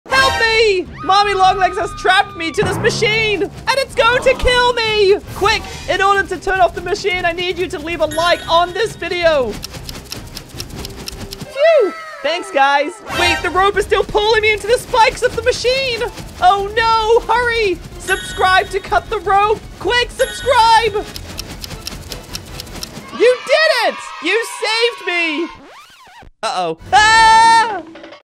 QUICK! Cut the ROPE in Minecraft! (Cartoon Animation)
Mommy Long Legs has captured Gizzy Gazza! QUICK cut the rope to save me!!

✨ CREDIT:
Animator: Moobulated